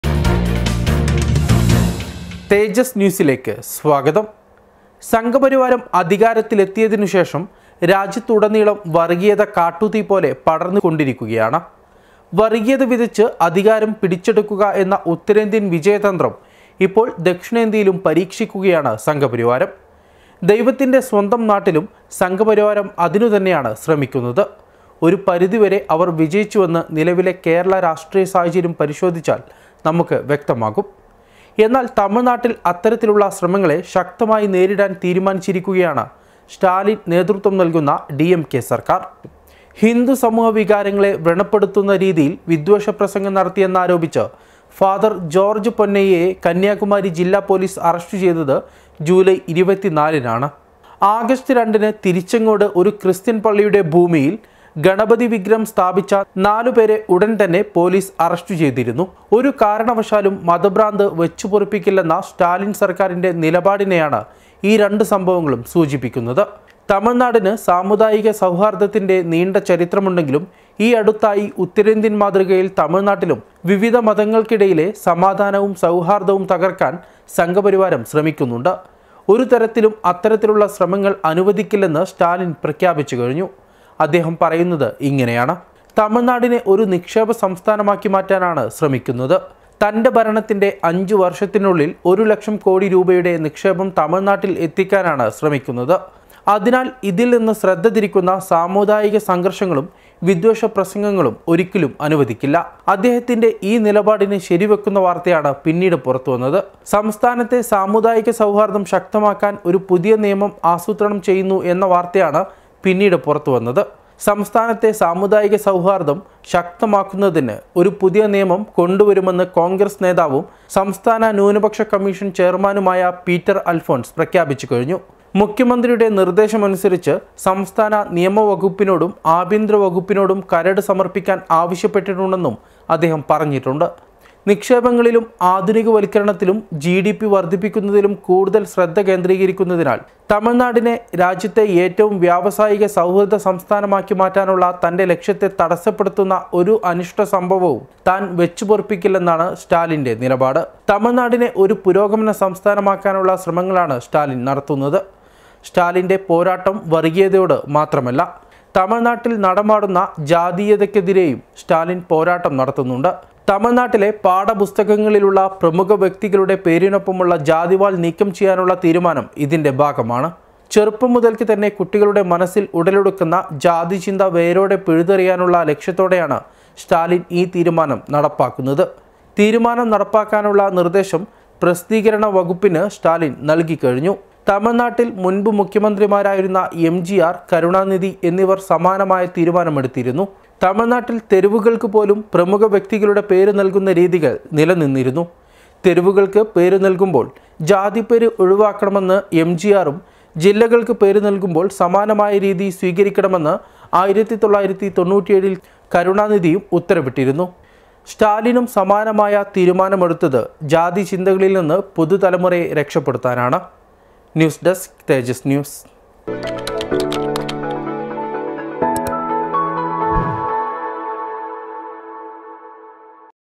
'ജാതിക്കളിയുമായി തമിഴ്‌നാട്ടിൽ വരേണ്ട' |THEJAS NEWS
തമിഴ്‌നാട്ടിൽ ഒരുതരത്തിലുള്ള വർഗീയ-ജാതീയ ശക്തികളേയും അനുവദിക്കില്ലെന്ന് പ്രഖ്യാപിച്ചിരിക്കുകയാണ് സ്റ്റാലിൻ സർക്കാർ. അത്തരക്കാരെ ശക്തമായി നേരിടാൻ തീരുമാനിച്ചിരിക്കുകയാണ് സ്റ്റാൻലിൻ നേത്യത്വം നൽകുന്ന ഡിഎംകെ സർക്കാർ